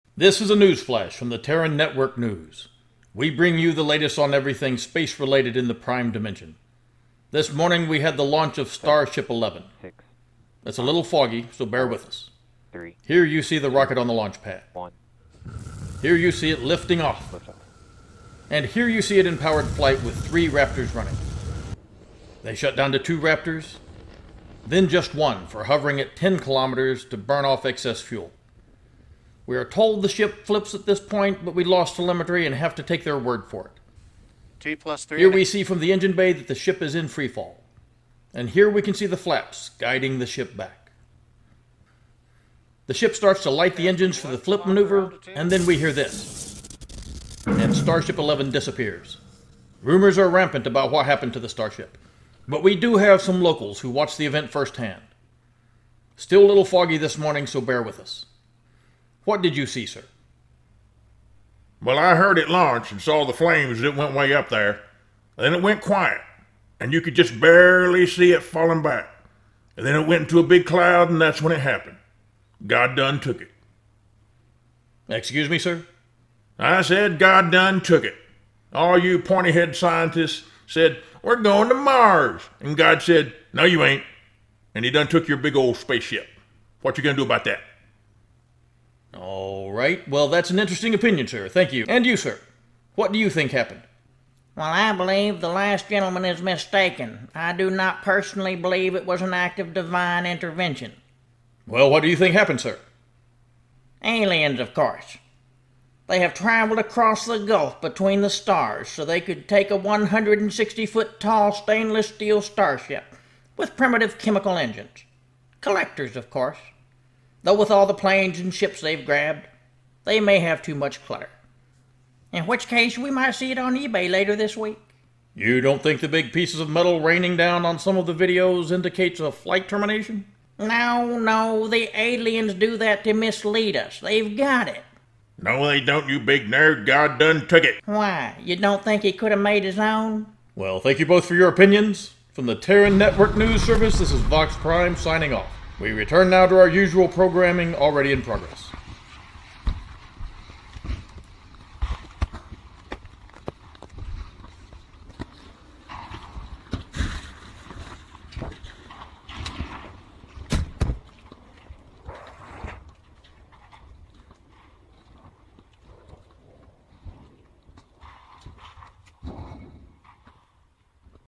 Starship Comedy
Exclusive Update on Starship 11 with South Texas Interviews!

Credit LabPadre, NASASpaceflight, Bocachicagal, SpaceX,
Ventron and the backup dancers,
and some very brave dude with a camera in South Padre!